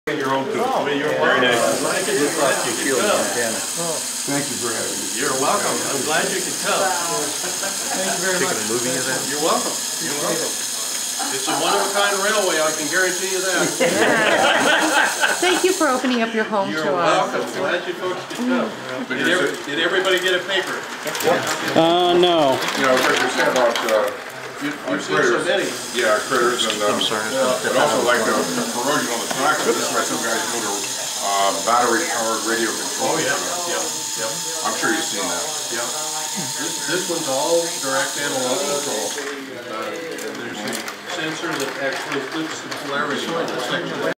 The Wilson's Railroad bridge
A bascule bridge in the living room on a G-Gauge semi-permanent carpet layout.